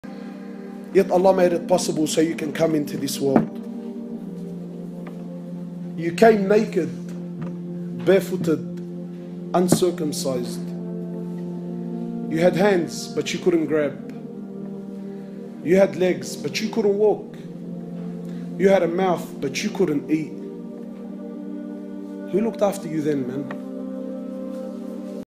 The Sustenance of Allah
When you were in your mothers womb, who was taking care of you. Ask yourself: When I was helpless who was there to help me? Gain a deep understanding of the sustenance of Allah with all that he has provided for us even when we were unborn and needed a way of living.